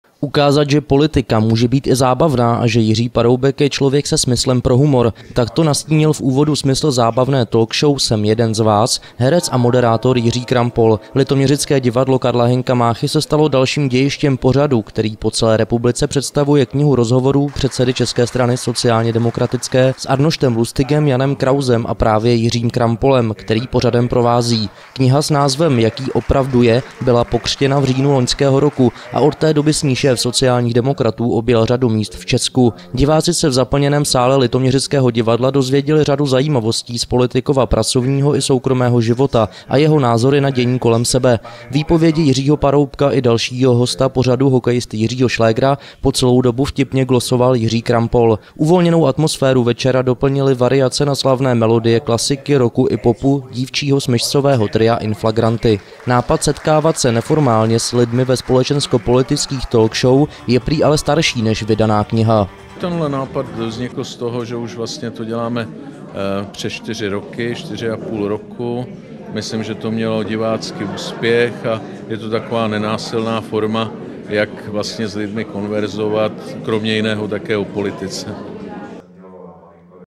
Talk show s Jiřím Paroubkem v litoměřickém divadle
Ukázat, že politika může být i zábavná a že Jiří Paroubek je člověk se smyslem pro humor. Takto nastínil v úvodu smysl zábavné talk show "Jsem jeden z vás" herec a moderátor Jiří Krampol. Litoměřické divadlo K. H. Máchy se stalo dalším dějištěm pořadu, který po celé republice představuje knihu rozhovorů předsedy ČSSD...